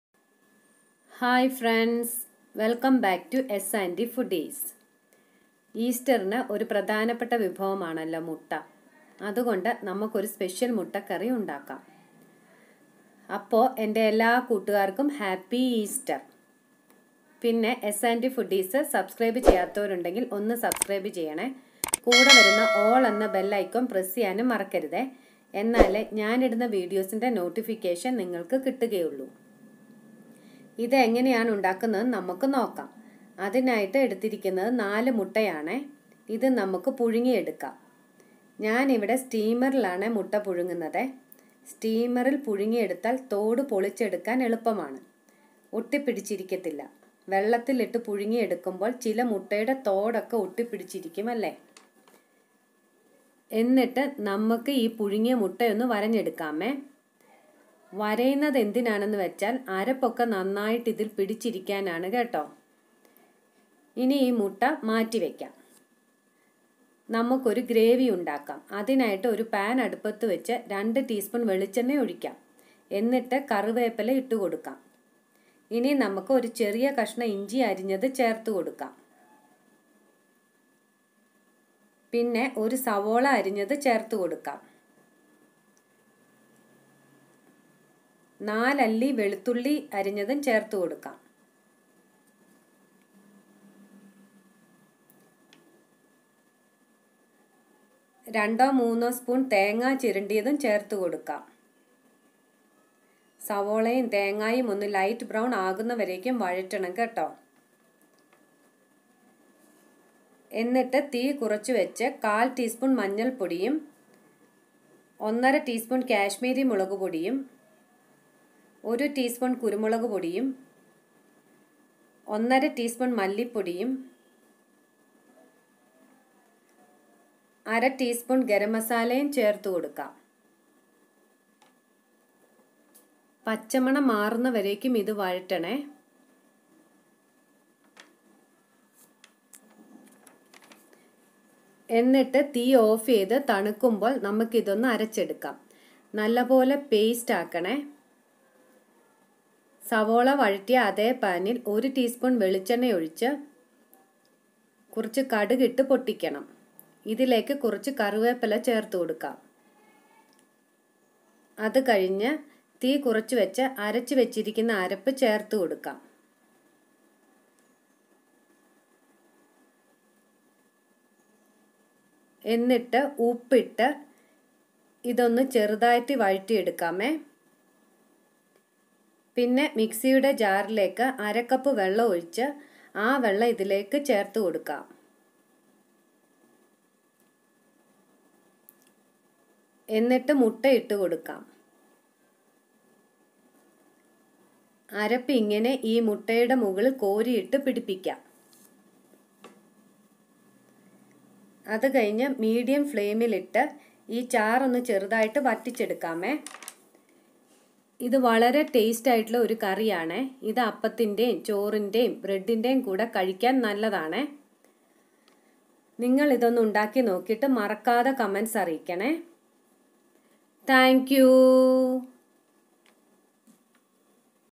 Egg Curry ||Egg Masala curry ||Egg recipe||S &T foodies
Ingredients
egg-4
oil- 2 tsp
curry leaves
ginger - small piece
onion- 1
garlic - 4
grated coconut - 2 - 3 tsp
mustard
curry leaves